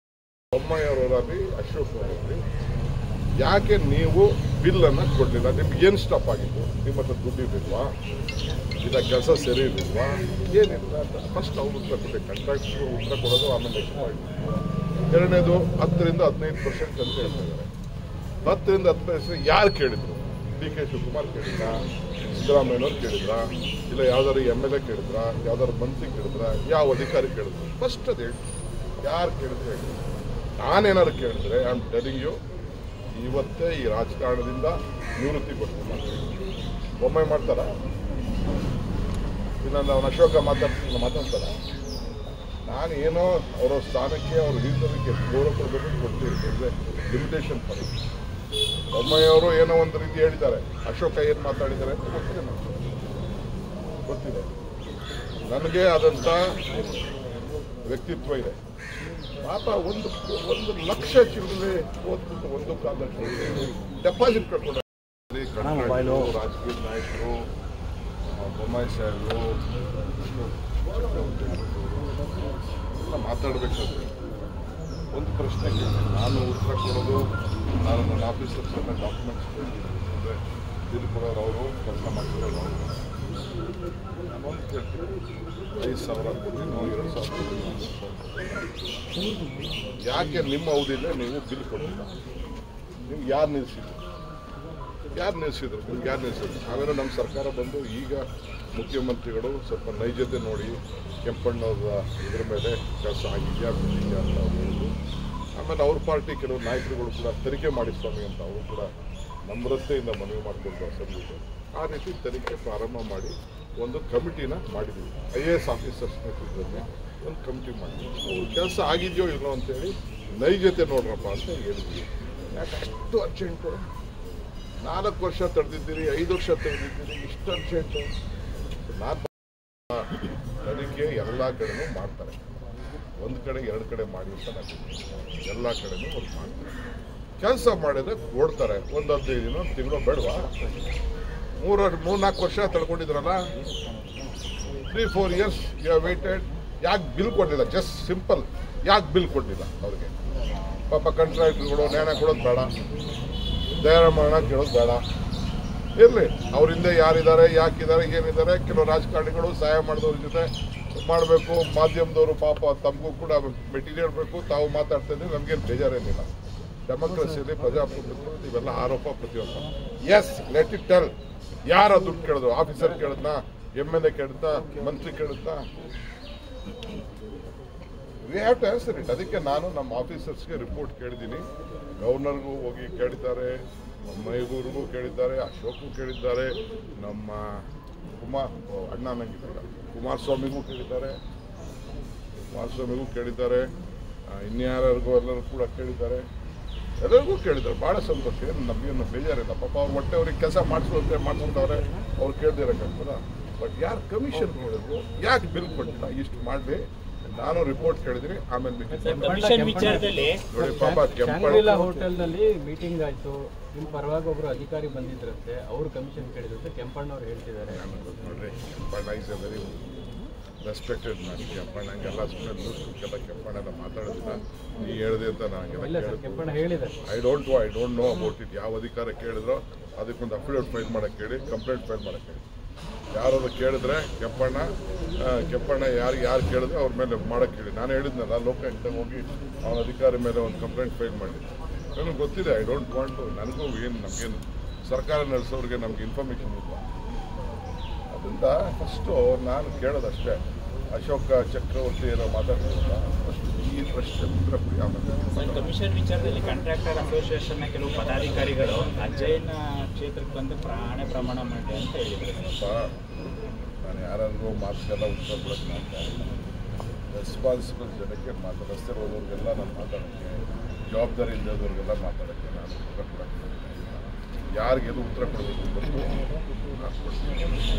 ನಾನು ಏನಾದರೂ ಕಮಿಷನ್ ಕೇಳಿದ್ರೆ , ಇವತ್ತೆ ರಾಜಕೀಯ ನಿವೃತ್ತಿ ಕೊಡ್ತೇನೆ , ಬೊಮ್ಮಾಯಿ ಆಗ್ತಾರಾ? ಅಶೋಕ ಆಗ್ತಾನಾ..?
10ರಿಂದ 15 ಪರ್ಸೆಂಟ್ ಕಮಿಷನ್ ಕೇಳಿದ್ರು ಅಂತಾರೆ ಯಾರು ಕೇಳಿದ್ರು?
ನಾನು ಏನಾದರೂ ಕೇಳಿದ್ರೆ , ಇವತ್ತೆ ರಾಜಕೀಯ ನಿವೃತ್ತಿ ಆಗುವೆ, ಬೊಮ್ಮಾಯಿ ಆಗ್ತಾರಾ ? ಅಶೋಕ ಆಗ್ತಾನಾ. ? ಎಂದ ಡಿಕೆಶಿ ಮಾತು ಮುಂದುವರಿಸಿ ಅಶೋಕ ಮಾತಾಡಿದ್ದು ನೋಡಿದ್ದೇನೆ , ಒಂದು ಕಾಲು ಲಕ್ಷ ಓಟಲ್ಲಿ ಸೋತು ಡೆಪಾಸಿಟ್ ಕಳ್ಕೊಂಡು ಇನ್ನೇನು ಆಗಬೇಕು? ಅಂತ ಅಶೋಕ್ ವಿರುದ್ಧ ಏಕವಚನದ ವಾಗ್ಗಾಳಿ ನಡೆಸಿದ್ರು.